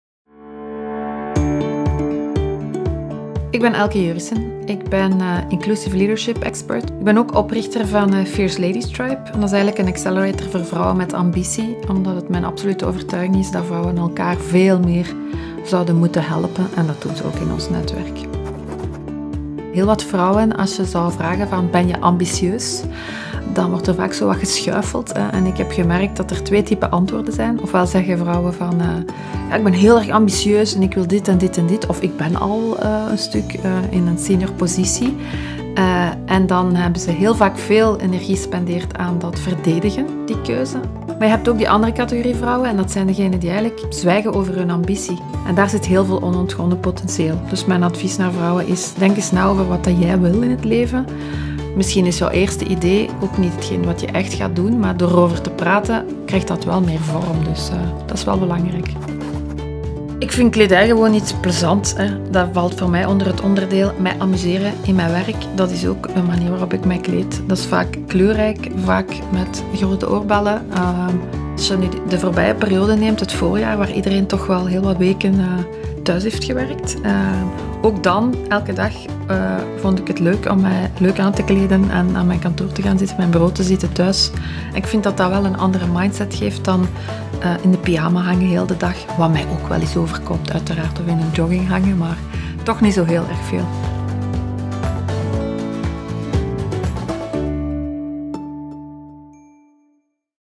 MAY Talks - Elke Jeurissen (NL)
Efficiënt, ambitieus en met de nodige vrouwelijke intuïtie. Deze drie straffe Belgische dames veroveren de wereld en effenen ondertussen het pad voor andere vrouwelijke ondernemers.
Expert Inclusive Leadership & Entrepreneur. Met haar project Fierce Ladies wil ze vrouwen met ambities inspireren en begeleiden.
“Wanneer ambitieuze vrouwen zich laten omringen door andere VROUWEN MET AMBITIE, dan zullen ze elkaar vooruitduwen.”